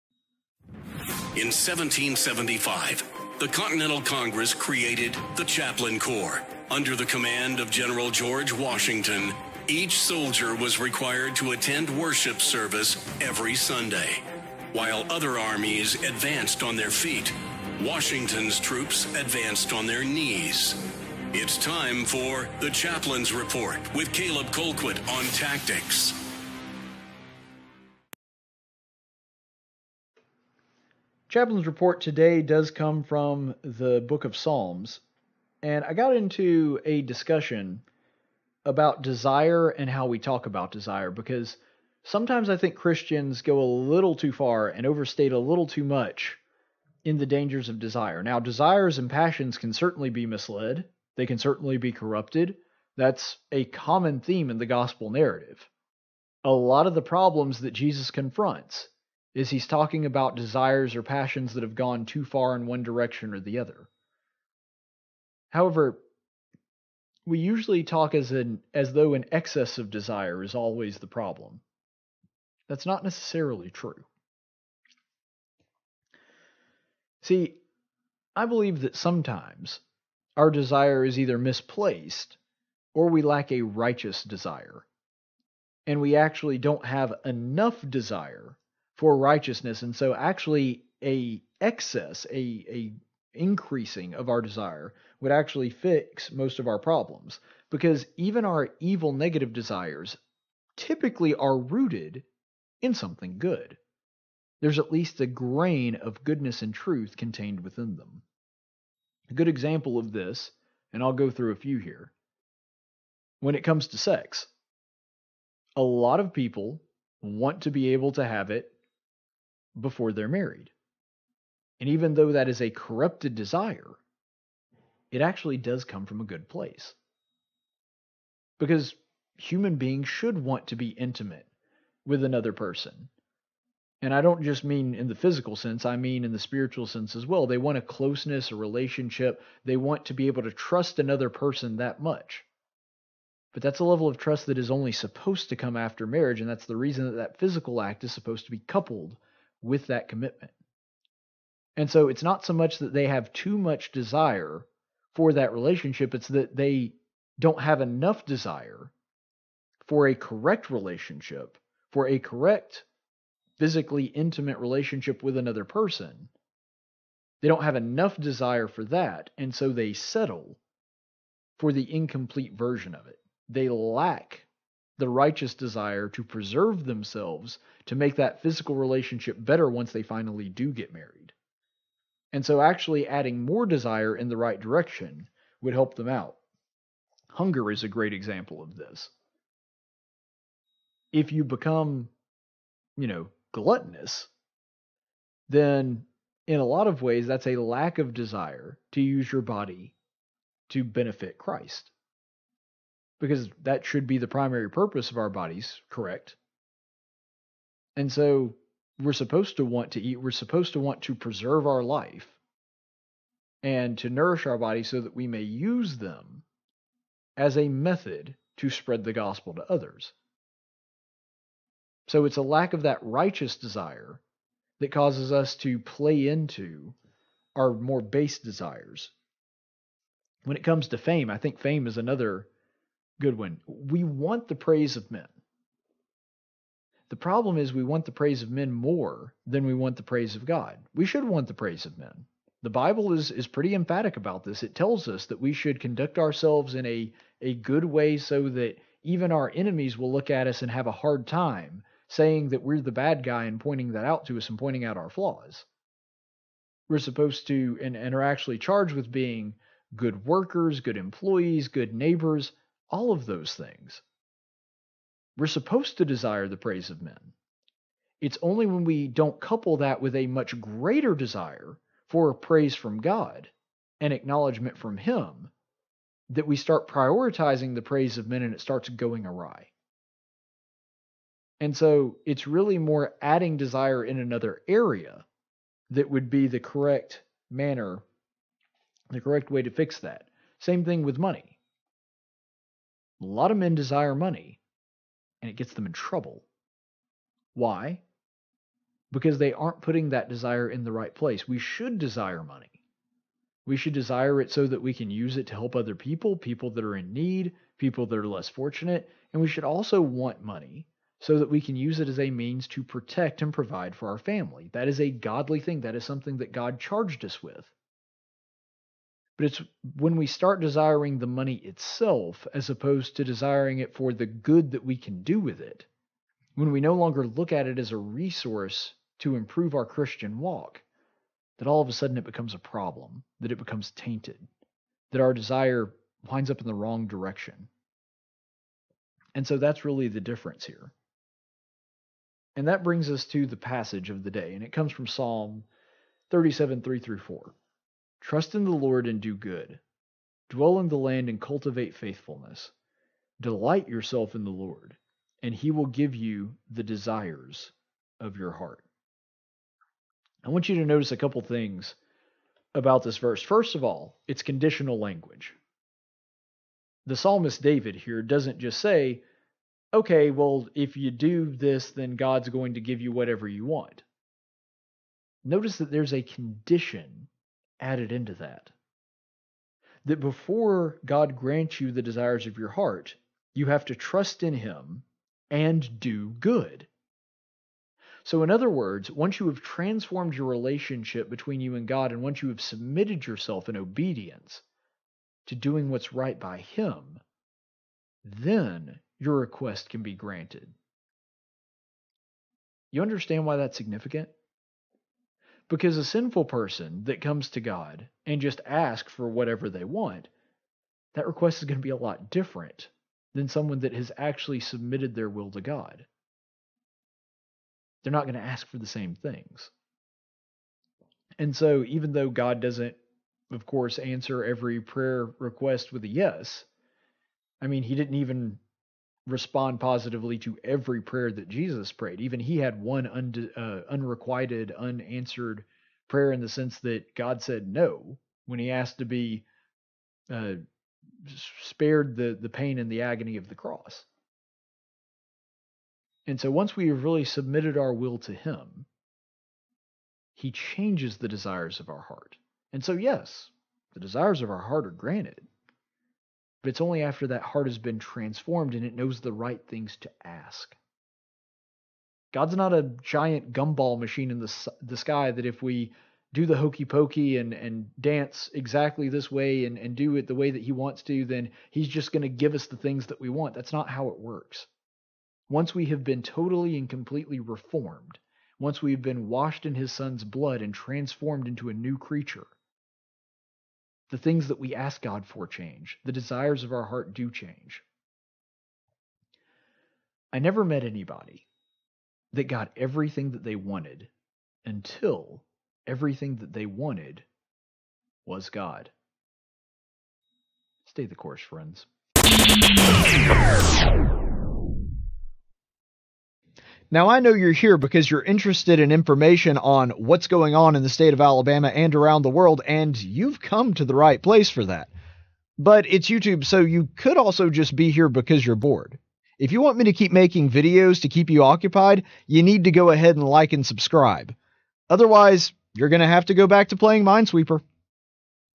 Chaplain's Report- Desire
David discusses God's willingness to grant our desires in Psalm 37:3-4 and the conditions that come with it.